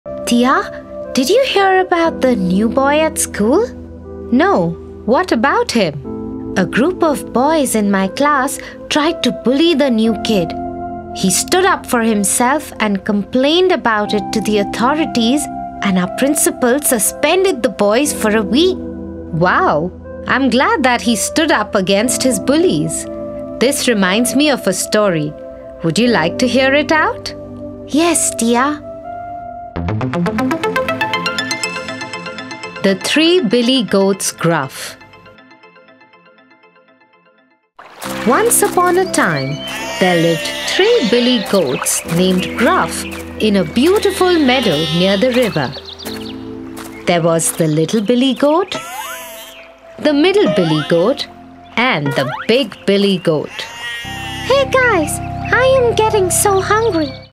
THE THREE BILLY GOATS GRUFF | Tia & Tofu | Learning Lesson for Kids | Link 👇👇
Kids Hut family presents "THE THREE BILLY GOATS GRUFF" by KIDS HUT.

✿ Voice Credit: Tulsi Kumar
✿ Script Credit: Sumra Khan